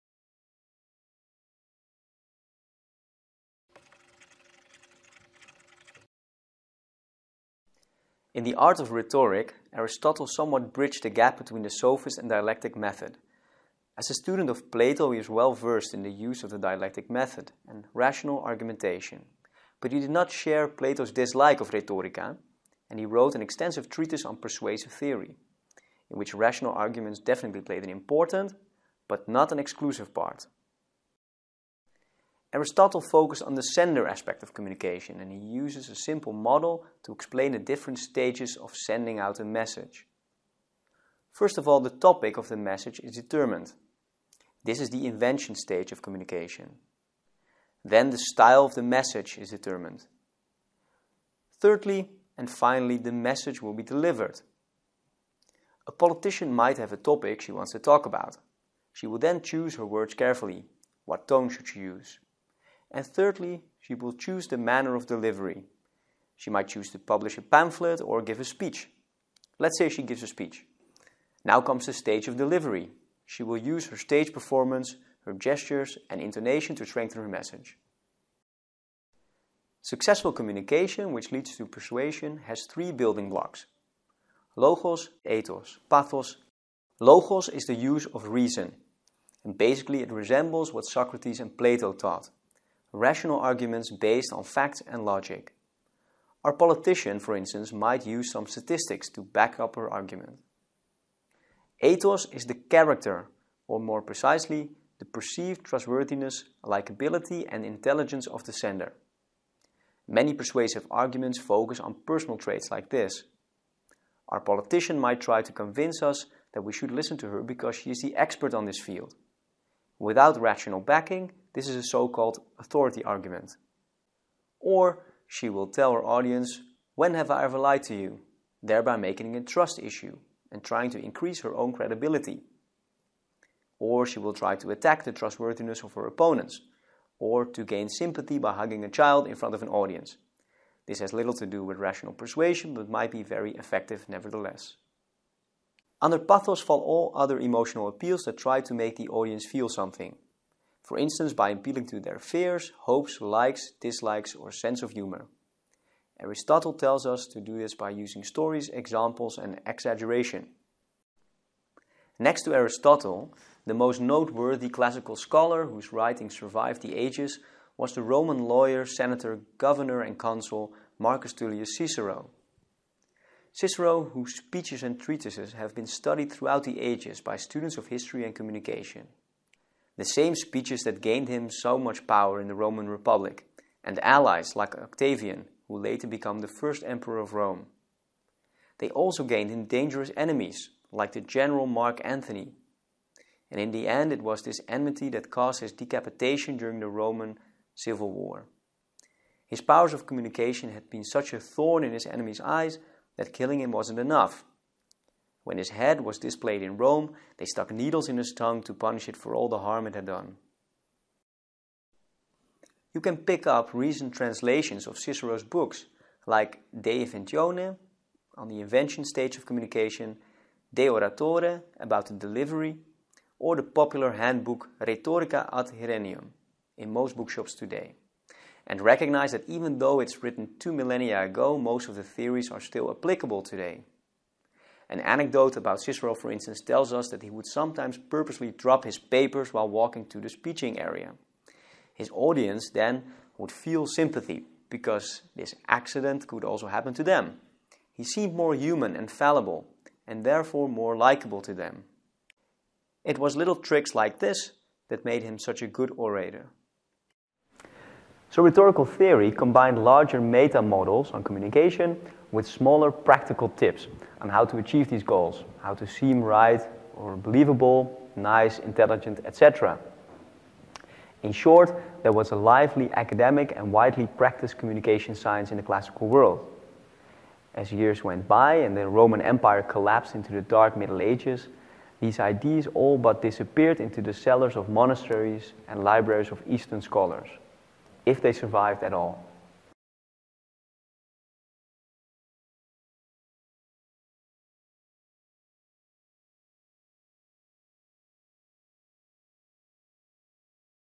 2.4 Rhetorical Theory
I'll explain some basic rhetorical models of communication by Aristotle and Cicero.

This clip is part of an 8 weeks free online course 'Introduction to Communication Science' given by the University of Amsterdam.